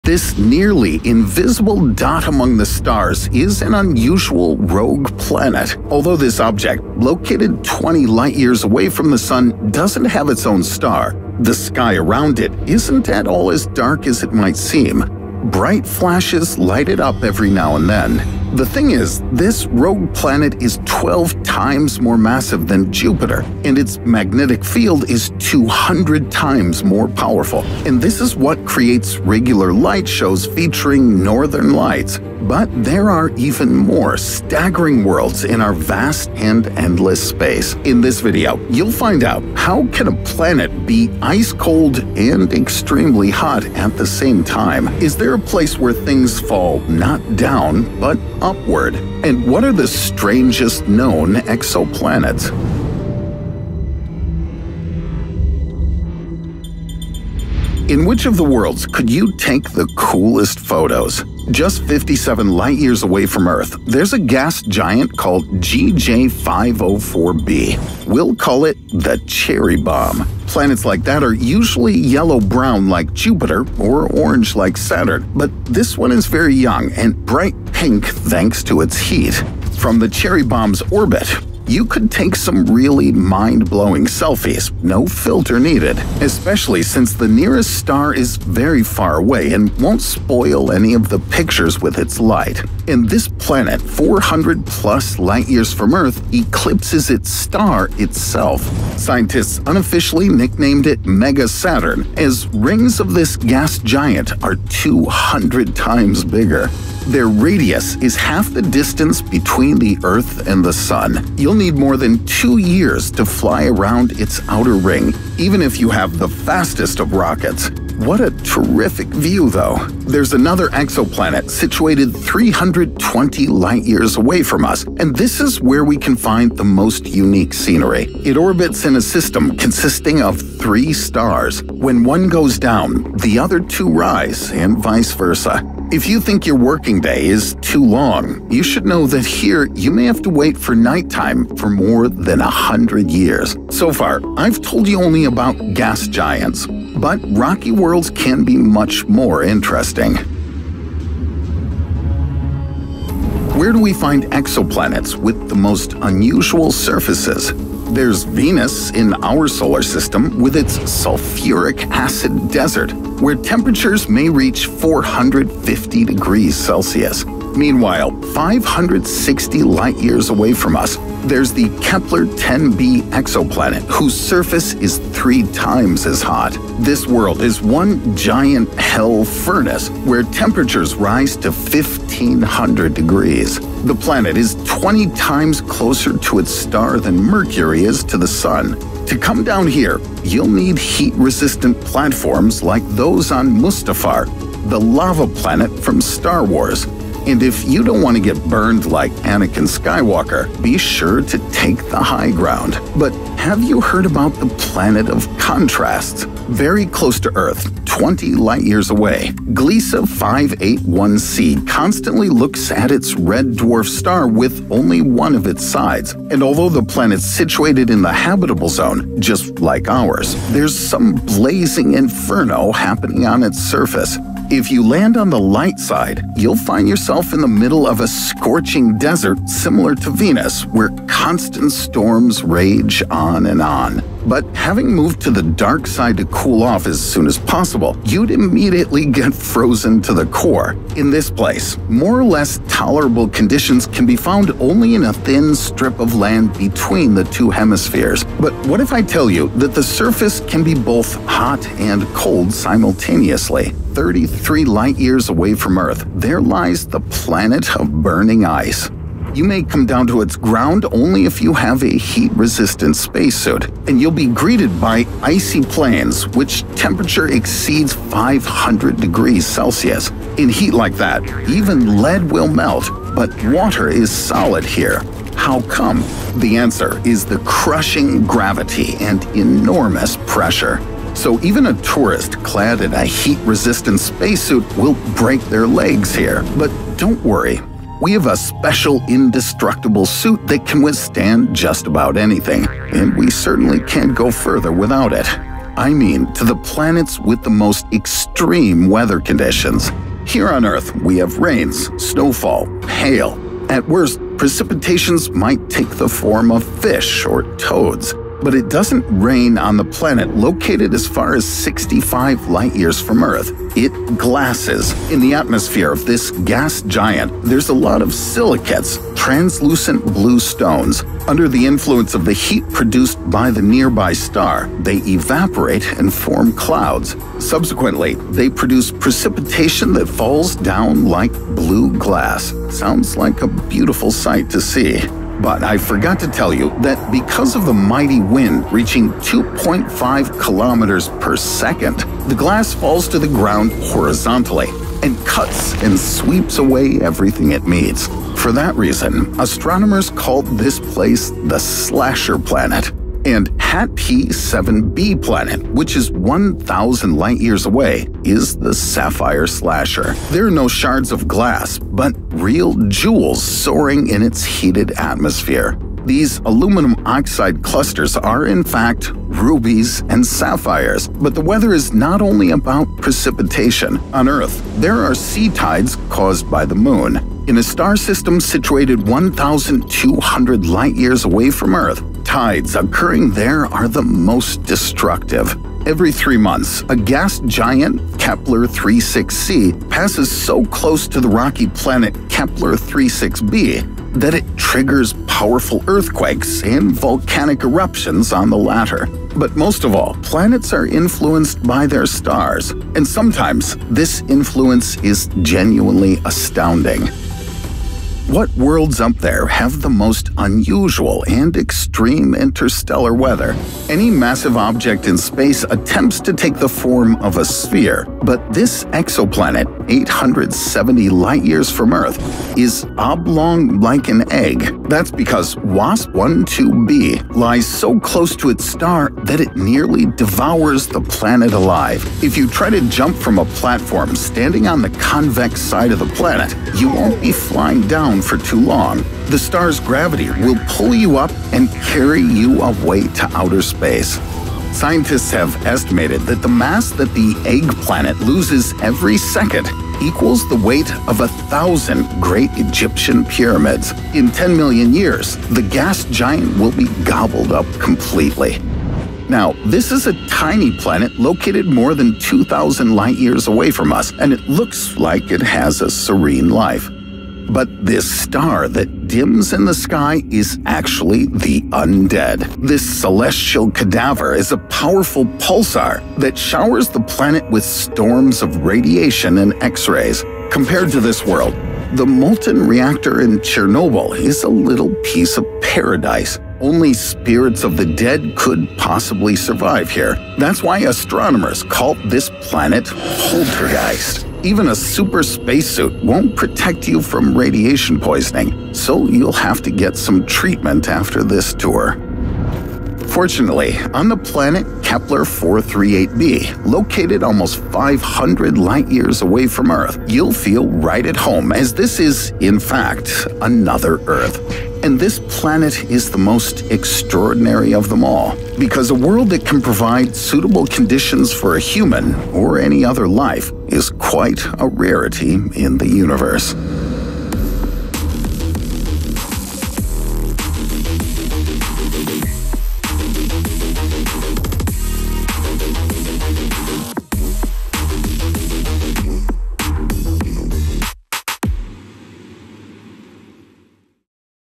Scientists Have Found Most Horrible Planet In Known Universe
This nearly invisible dot among the stars is an unusual rogue planet. Although this object located twenty light-years away from the Sun doesn’t have its own star, the sky around it isn’t at all as dark as it might seem. Bright flashes light it up every now and then. The thing is, this rogue planet is twelve times more massive than Jupiter, and its magnetic field is two hundred times more powerful!
And this is what creates regular light shows featuring northern lights! But there are even more staggering worlds in our vast and endless space.
In this video, you’ll find out: how can a planet be ice cold and extremely hot at the same time? Is there a place where things fall not down but upward?
And what are the strangest known exoplanets?